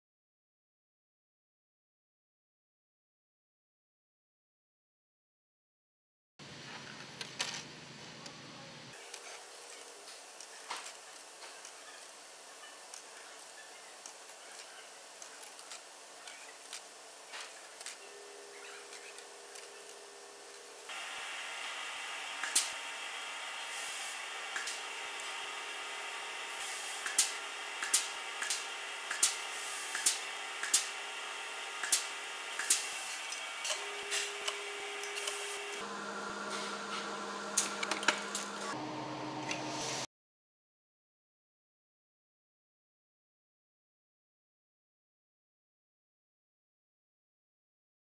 Manufacturing A Bulldog Springbok Rake
This video demonstrates the processes used to manufacture a Bulldog Springbok Lawn Rake. This is the most popular lawn rake made by Bulldog Tools. Ideal for scarifying lawns and removing unwanted growth.